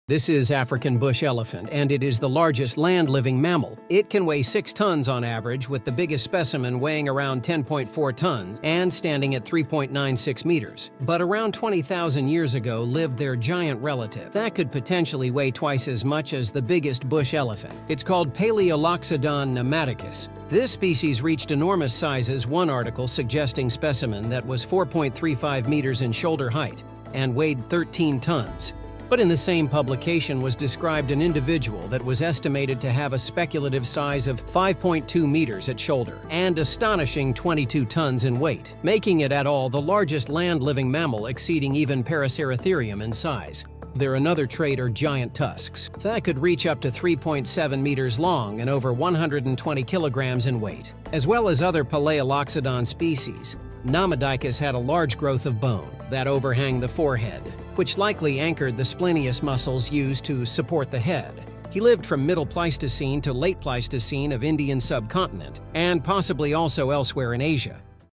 Biggest elephant ever... Palaeoloxodon namadicus.
- This is African bush elephant and it is the largest land living mammal. It can weigh 6 tonnes (13,200 lb) on average with the biggest specimen weighing around 10.4 tonnes (23,000 lb) and standing at 3,96 meters (13.0 ft). But around 30 thousand years ago lived their giant relative that could potentially weigh twice as much as the biggest bush elephant. It’s called Palaeoloxodon namadicus. This species reached enormous sizes one article suggesting specimen that was 4.35 meters (14.3ft) in shoulder height and weighed 13 tonnes (29,000 lb). But in the same publication was described an individual that was estimated to have a speculative size of 5.2 meters (17.1 ft) at shoulder and astonishing 22 tonnes (49,000 lb) in weight making it at all the largest land-living mammal exceeding even Paraceratherium in size. There another trait are giant tusks that could reach up to 3.66 metres (12.0 ft) long and over 120 kilograms (260 lb) in weight. As well as other Palaeoloxodon species namadicus had a large growth of bone (the parieto-occipital crest) that overhangs the forehead which likely anchored the splenius muscles used to support the head. He lived from Middle Pleistocene to Late Pleistocene of  Indian subcontinent, and possibly also elsewhere in Asia and became extinct within the last 50,000 years with some records possibly as late as 25,000 years ago.